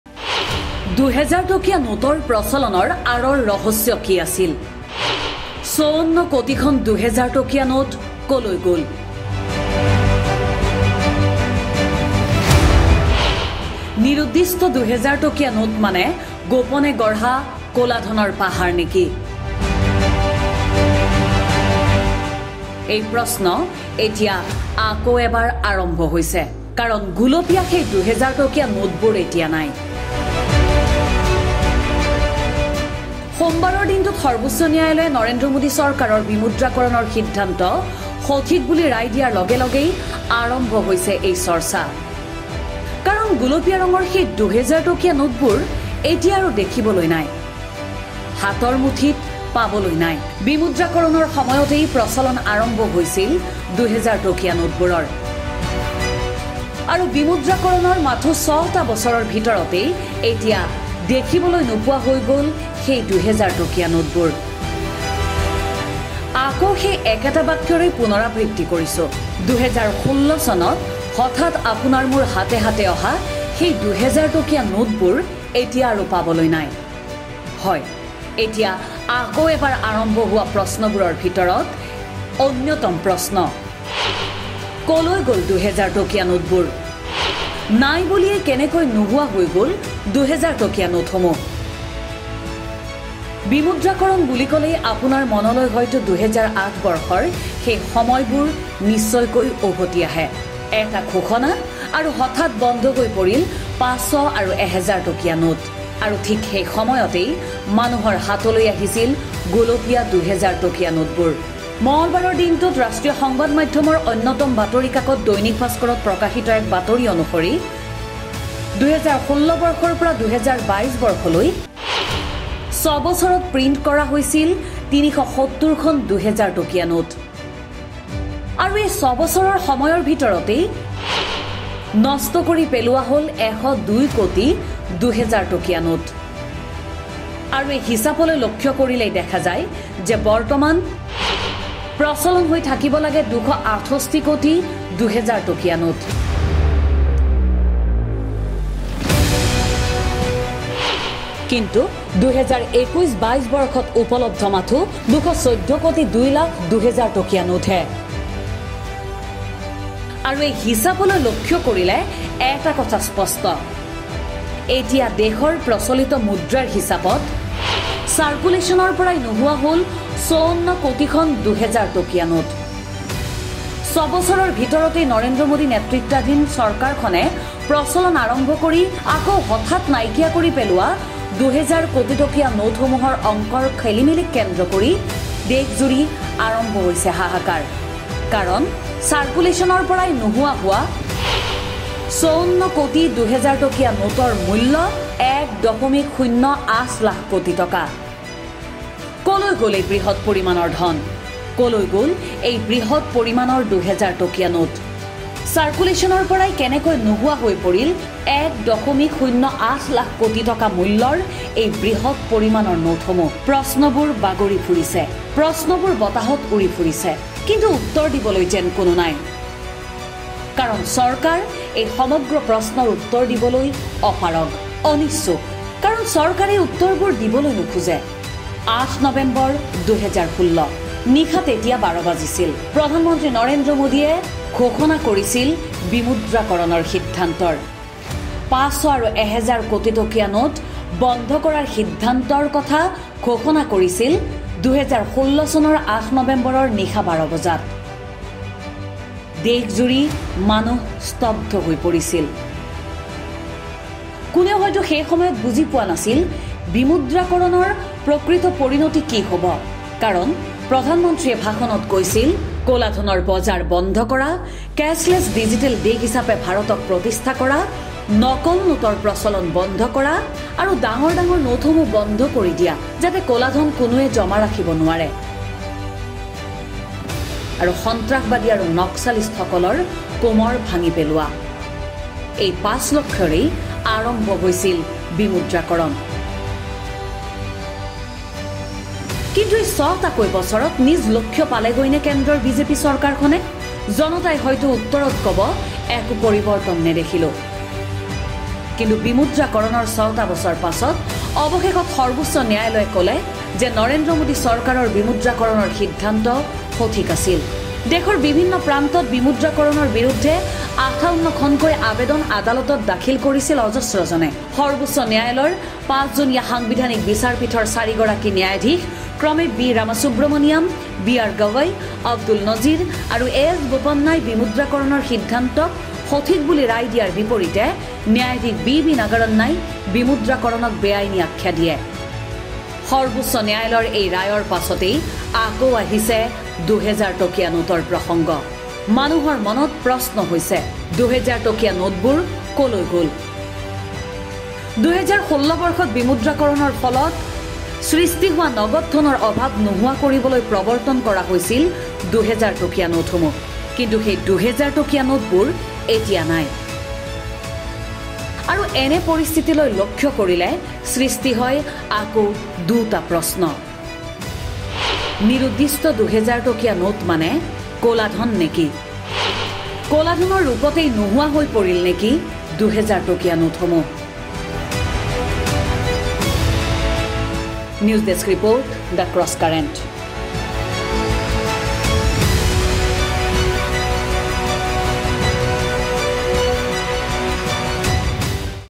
৫৪ কোটিখন ২০০০ টকীয়া নোট ক'লৈ গ'ল?
২০০০ টকীয়া নোটৰ প্ৰচলনৰ আঁৰৰ ৰহস্য কি আছিল? ৫৪ কোটিখন ২০০০ টকীয়া নোট ক'লৈ গ'ল?